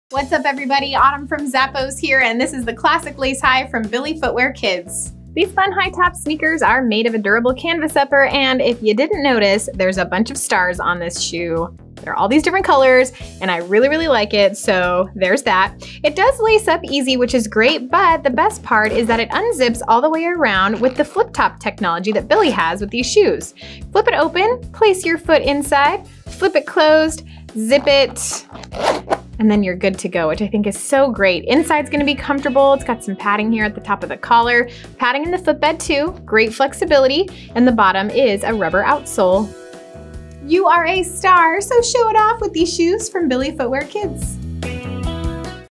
BILLY Footwear Kids Classic Lace High (Toddler/Little Kid/Big Kid) SKU: 9094538
View Billy Footwear Kids Size Chart
Get their cool style back-to-basics with the BILLY Footwear&reg; Kids Classic Lace High sneaker!
The BILLY Classic Lace High features innovative FlipTop technology, giving kids a fun and easy way to put on and take off their shoes.
Durable canvas upper in a high-top silhouette.
Wrap-around zipper closure. Flip! Zip! Go!
Rounded toe.
Rear pull-tab for easy entry.
Lace-up vamp offers a custom fit.
BILLY Goat logo embroidery on the side panel.
Breathable textile lining.
Well-cushioned footbed for all-day play.
Durable and flexible rubber outsole.
Convenient visual sizing confirmation on the inside flap.
Imported.